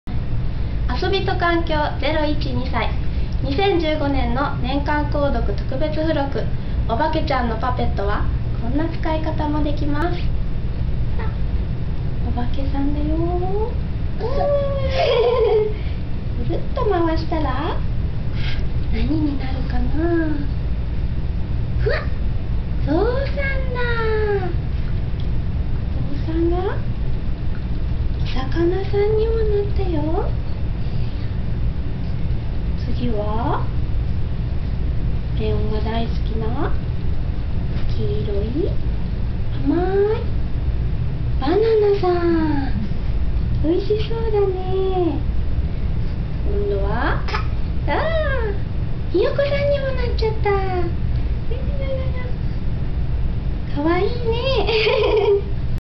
2015年「あそびと環境0.1.2歳」年間購読特別付録「おばけちゃんのばけばけパペット」あそび方
子どもとのふれあいあそびにぴったりの「おばけちゃんのばけばけパペット」は、園にお伺いしている学研特約代理店に年間購読をお申し込みいただいた方のみの特別付録です。